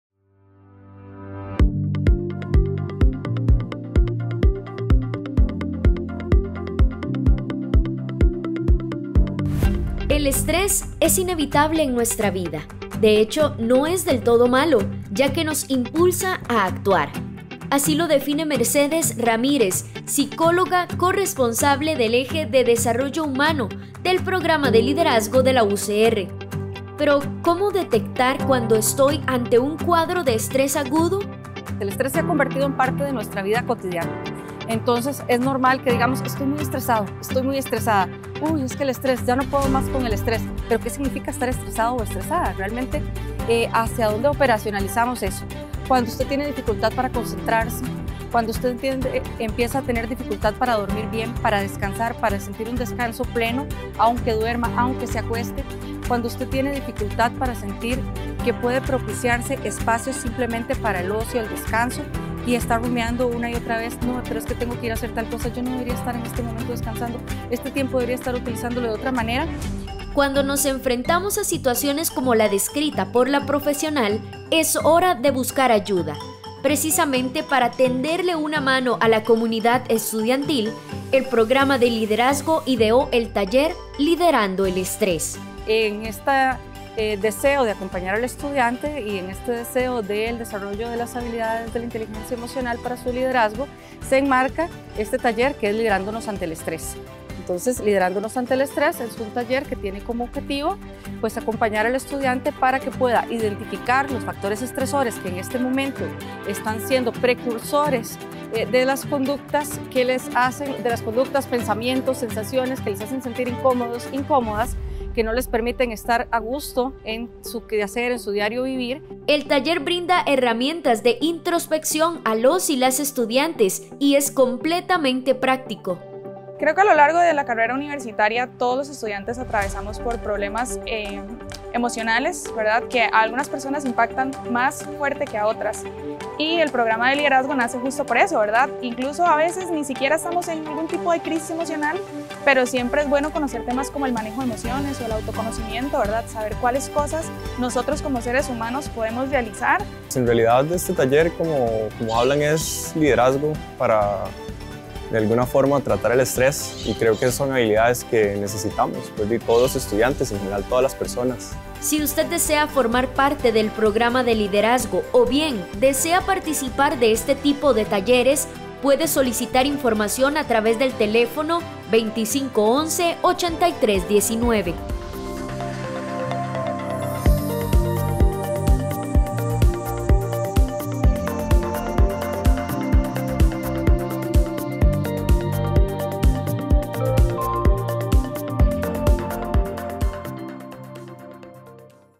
¿Cómo manejar el estrés? Un taller del Programa de Liderazgo le ayuda a responder esta interrogante
Se aproxima el fin de año, época en la que las labores académicas y las responsabilidades laborales crecen. Ante panoramas como este, el estrés se presenta en nuestra vida. ¿Cómo manejarlo adecuadamente? Le recomendamos que preste mucha atención a la siguiente nota.